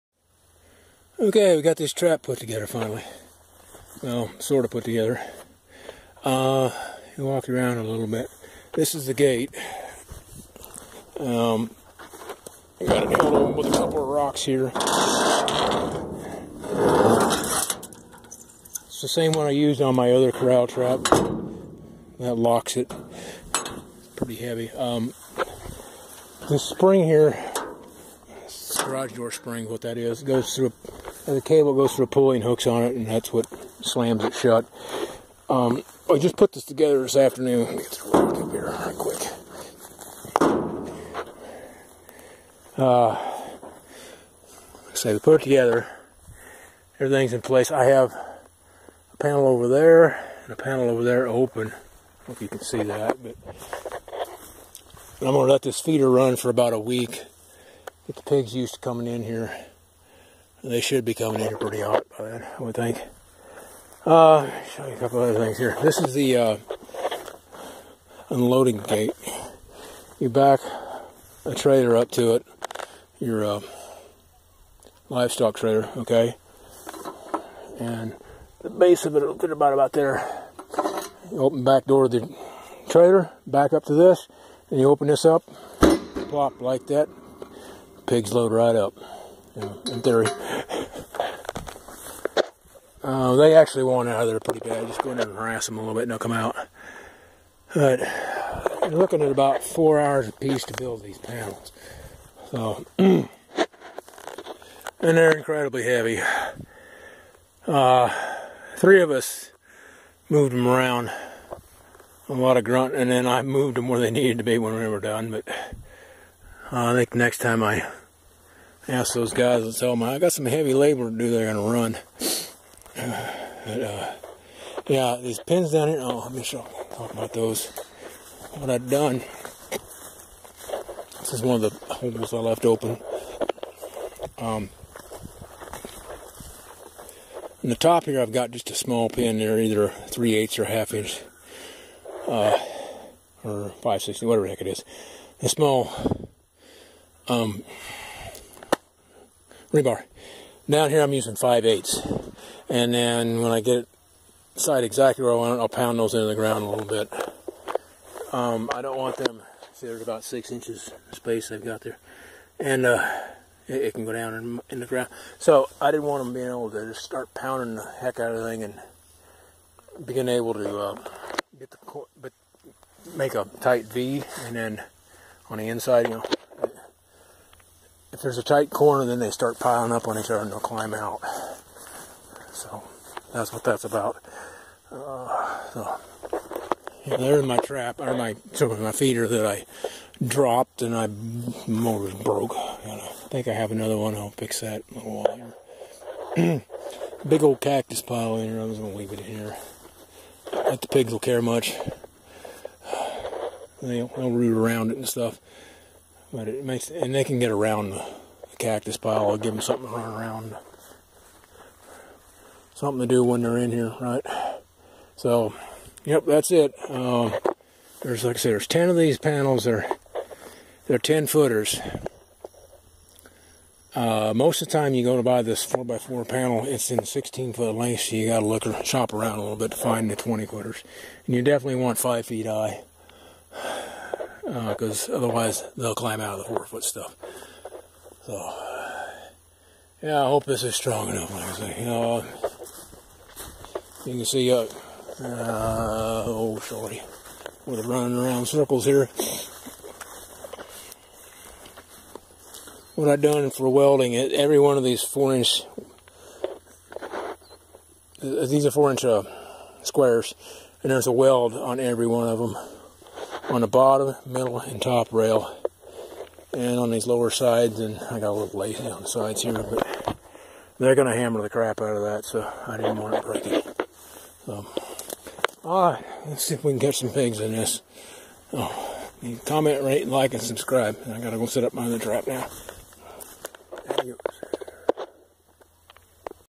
New Corral Trap Setup
My 3rd attempt at a feral hog corral trap.  This is the strongest one yet and is portable.  There are ten 10'x5' panels plus a 6' gate and a 3' loading chute.  The panels are made from 2 3/8" drill pipe and 4" utility panels.  The panels are welded every 4".  They weigh around 200lbs each.  One guy can set this up but it's a workout.  It's overkill but I don't want to have to be repairing this or loosing trapped hogs.

Since I set this up in Sept (4 months ago) I've only caught 3 hogs.  I only set the trap about once every 2 weeks.  The location doesn't have the hog traffic I thought it would.  Also we just don't have as many hogs on this ranch as we did a year ago.  Either my trapping efforts really are making a dent in the population or they've migrated elsewhere.